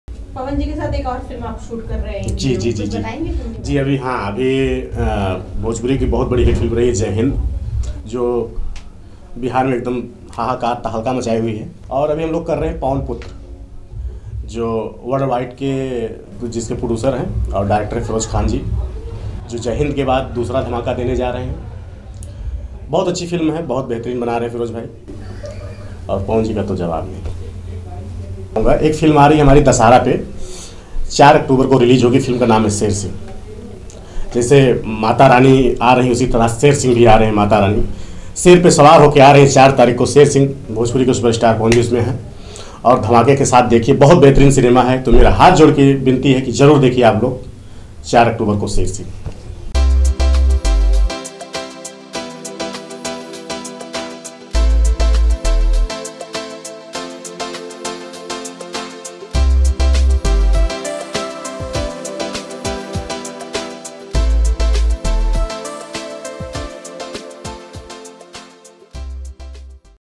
Pawan Singh की 'पवन पुत्र ' के बारे में Sanjay Verma ने क्या कहा ? | Lehren Bhojpuri
पवन सिंह इन दिनों फिल्म पवन पुत्र की शूटिंग कर रहे है.इस फिल्म के बारे में संजय वर्मा ने क्या कहा,देखिये वीडियो.